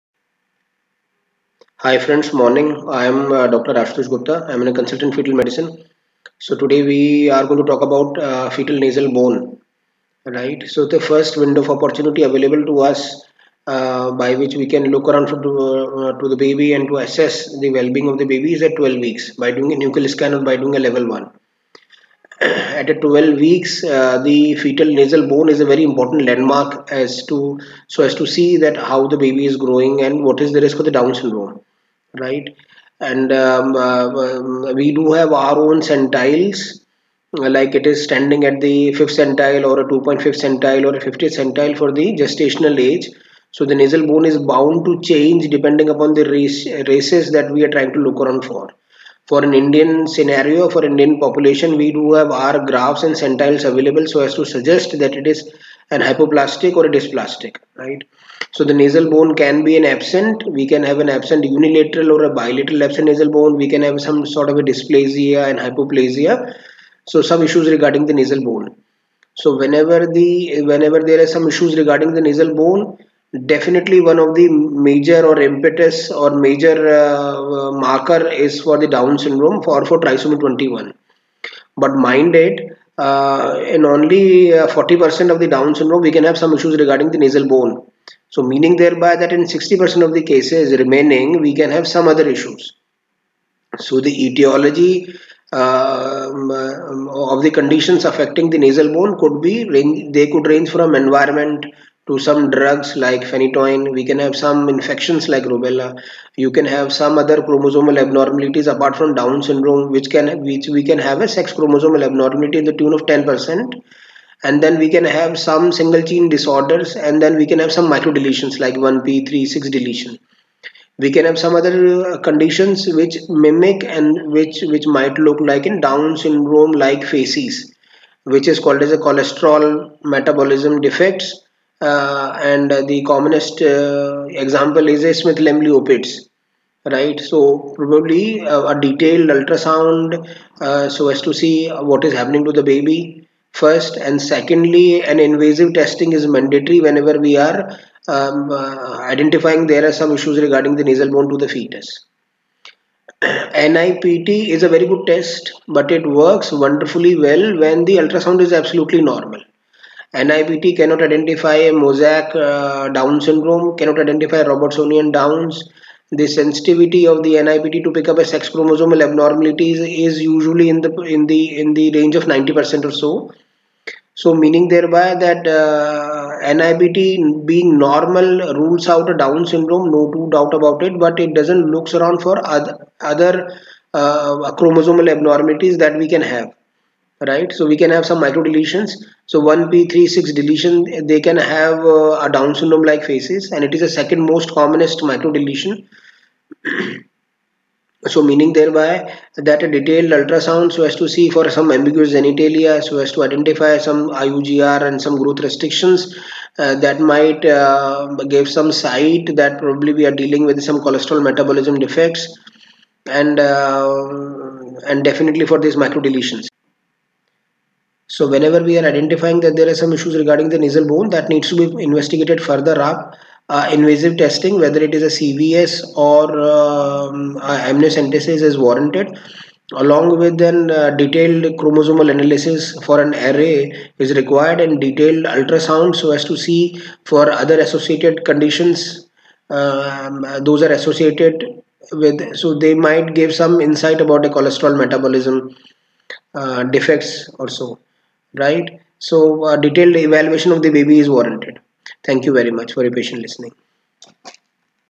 Nasal bone & Fetus
Absent nasal bone is major marker for fetal aneuploidy; Trisomy 21.
Hypoplasia of the nasal bone refers to fetal nasal bone appearing smaller by varying degrees; hypoplasia is a spectrum.
0.5-1.2% of normal (euploid) fetuses have been found to have a hypoplastic nasal bone on a routine 2nd trimester scan, compared to 43-62% of fetuses with Down syndrome
Down’s like facies can occur with exposure to some drugs (warfarin), infection (syphilis), other chromosomal abnormalities (Trisomy 18 – 50%, Trisomy 13 – 30%, Turner – 10%), Microdeletion – 1p36 deletion syndrome, Cholesterol metabolism defects – SLOS (Smith lemli opitz).
Hypoplasia of nasal bones seen on antenatal ultrasound warrants invasive testing (99.9999% accurate) with microdeletion study with detailed ultrasound to seen for other associated features.
This is being explained by Dr Ashutosh Gupta